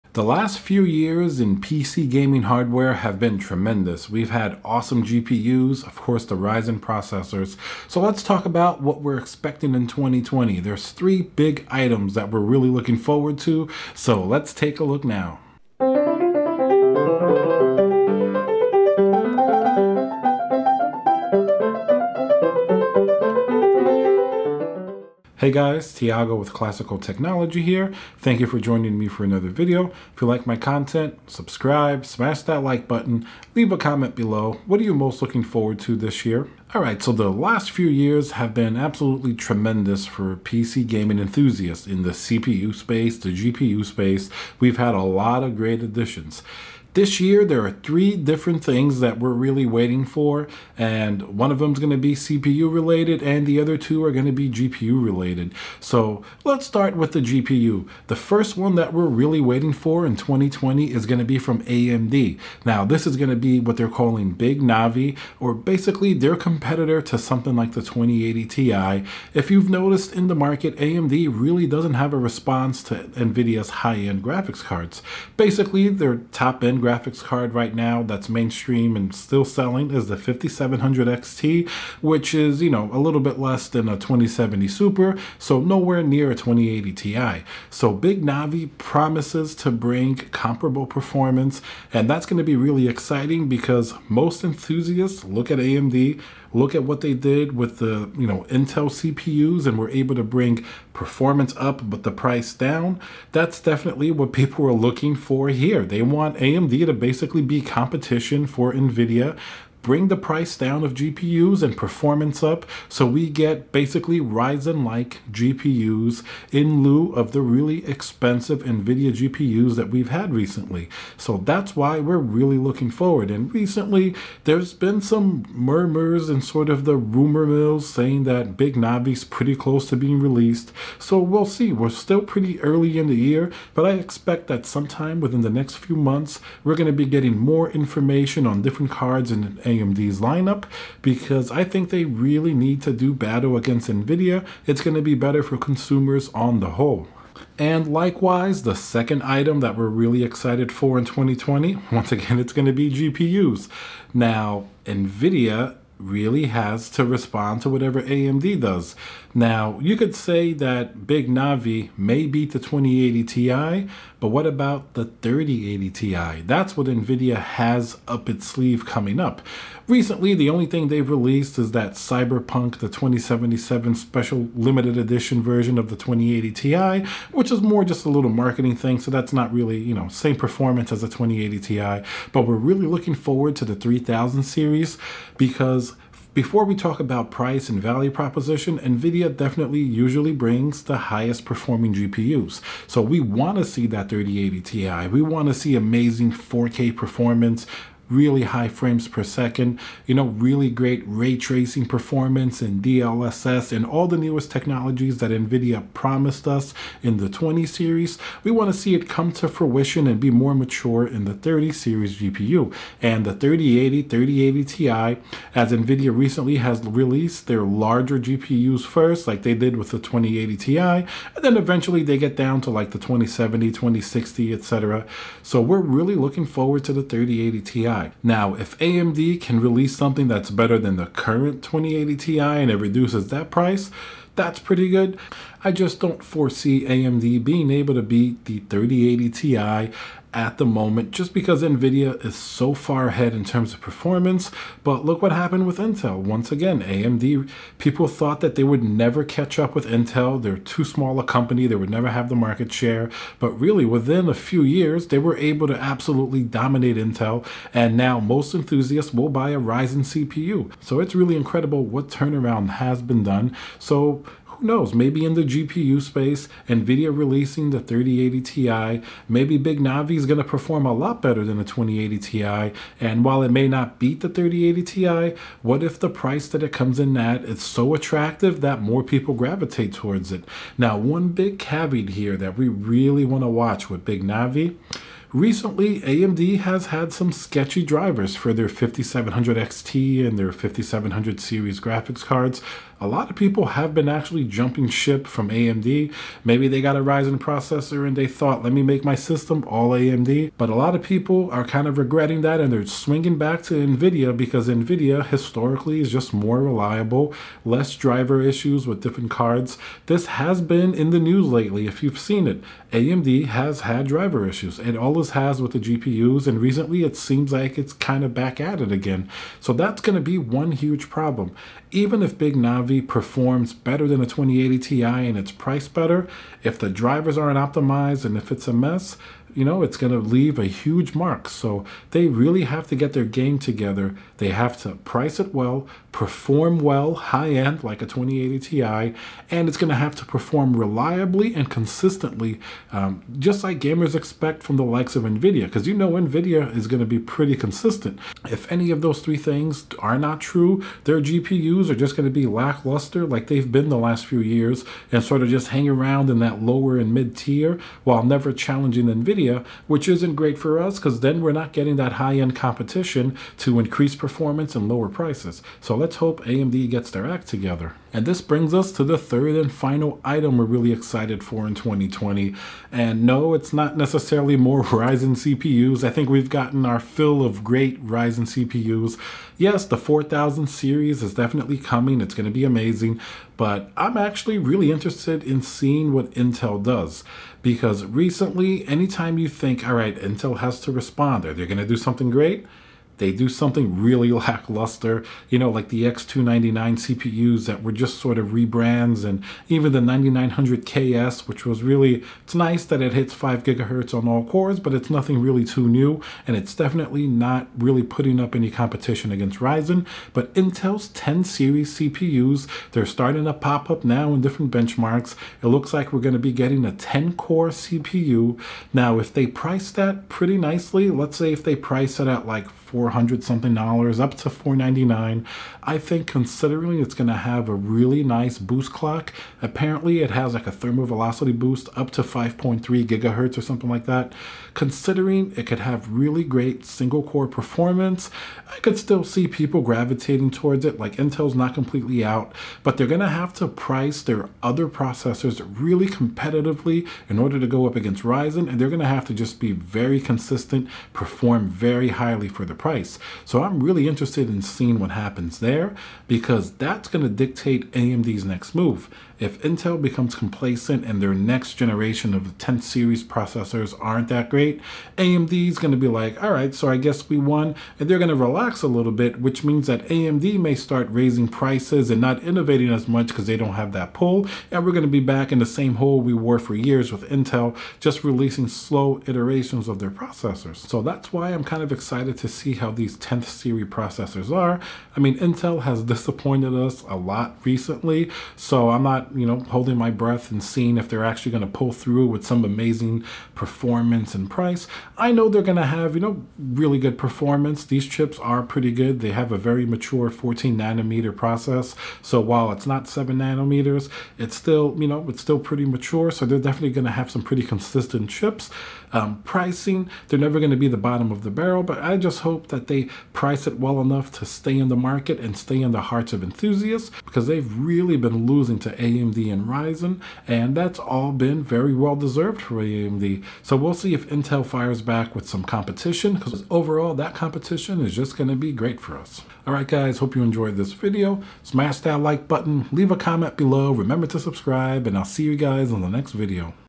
AMD Big Navi, Nvidia 3080 ti, and Intel 10 core 5.3ghz CPUs in 2020
Let's talk about what is coming in 2020, Big Navi from AMD, Nvidia 3080 ti and 3000 series GPUs, and Intel 10 Core mainstream cpu. Add Ryzen 4000 series to that, and we have a stellar year!